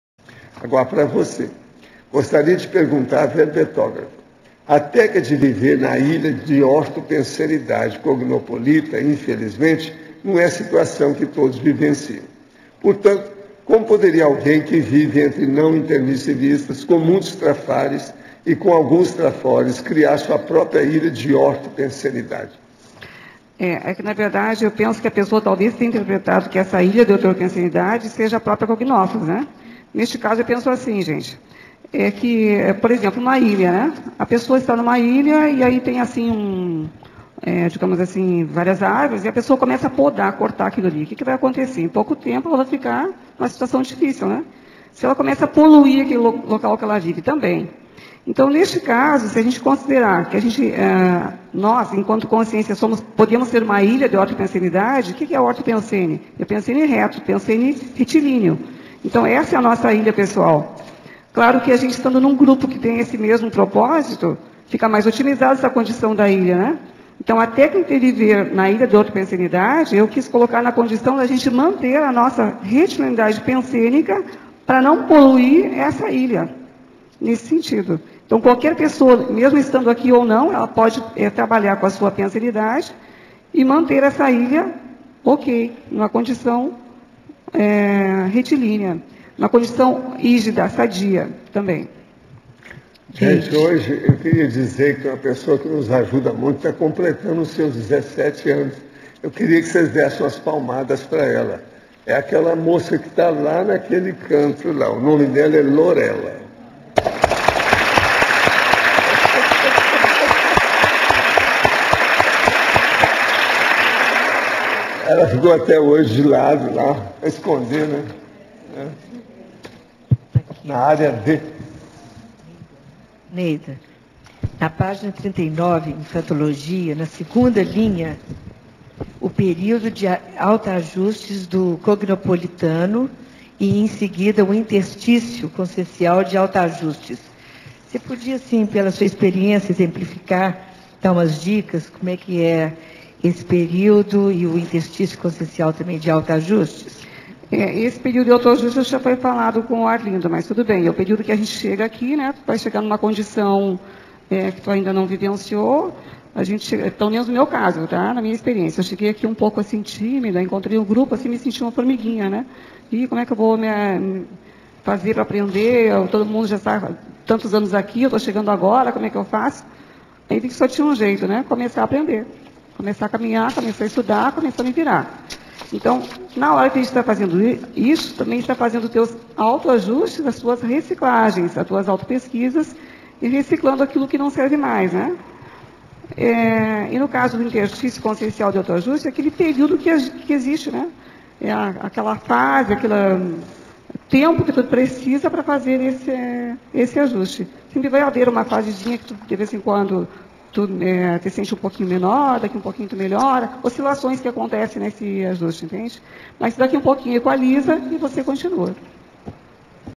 CONSCIENCIOLOGIA - Manter uma Ilha de ORTOPENSENIDADE - Autoajustes CONSCIENCIAIS
Tertúlia - Aula 1967 - Terça-feira - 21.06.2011 - Quinquênio cognopolita (Intrafisicologia) Ao vivo todos os dias de forma gratuita nesse horário, 12:30 às 14:30...Clique lá e fique mais sábio(a)... Tertúlias com o prof. Waldo Vieira (Evoluciologia)